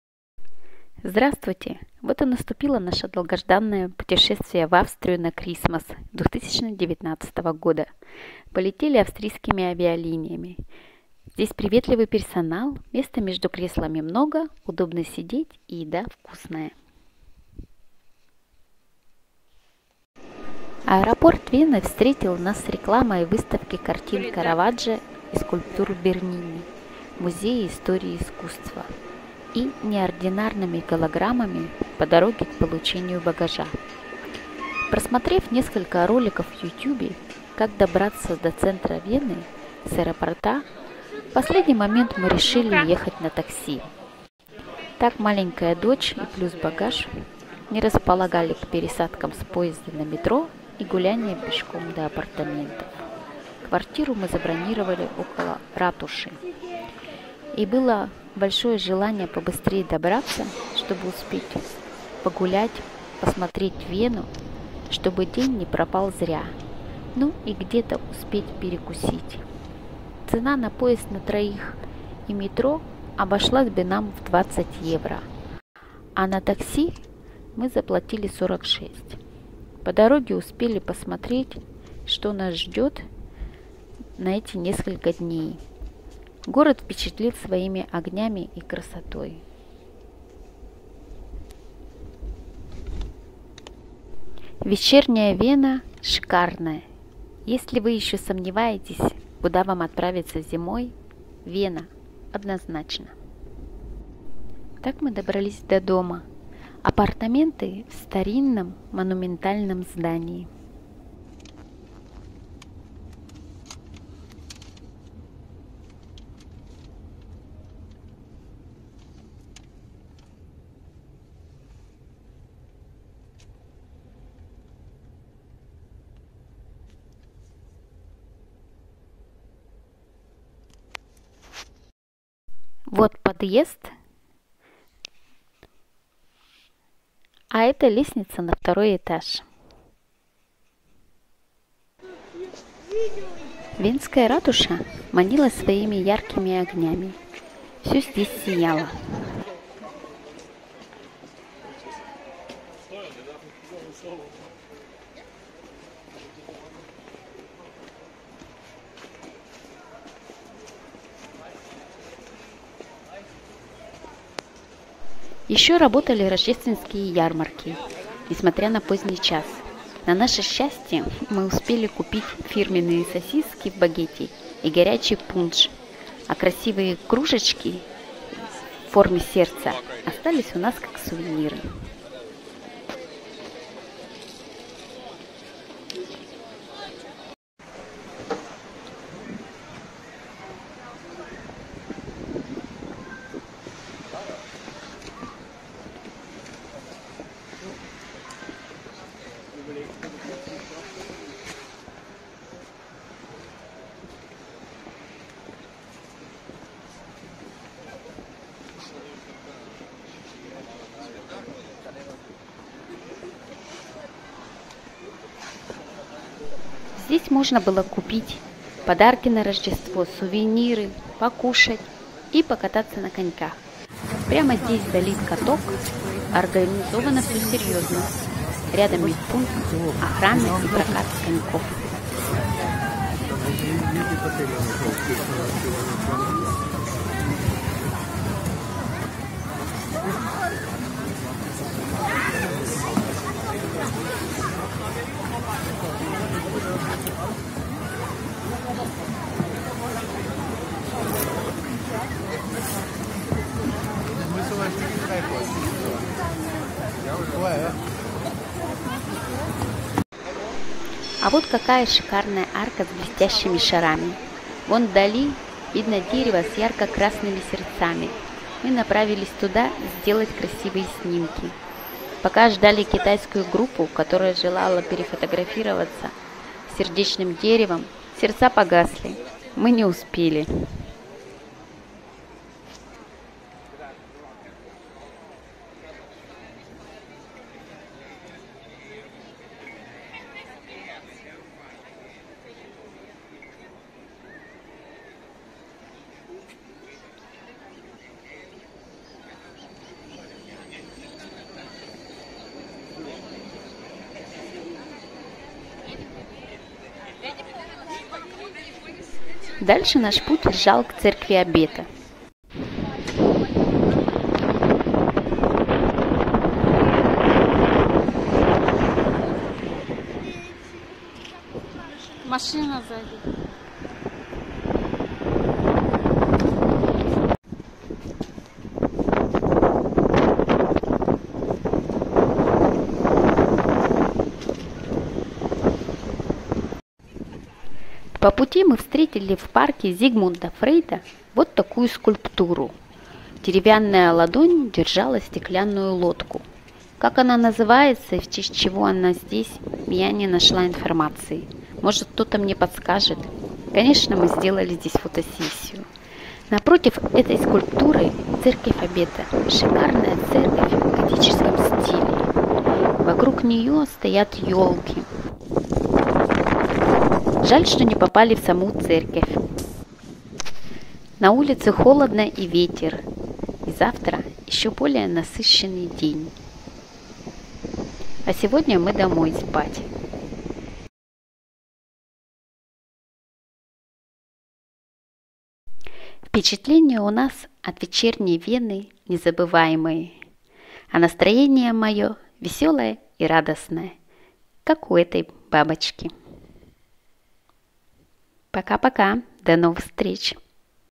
Austria Vienna Chrismas 2019. Австрия, Вена ,Рождество декабрь 2019 года
День 1. Прилет в Вену Austrian airlines. Рождественская Ратуша, Церковь Обета. Замечательно и красиво.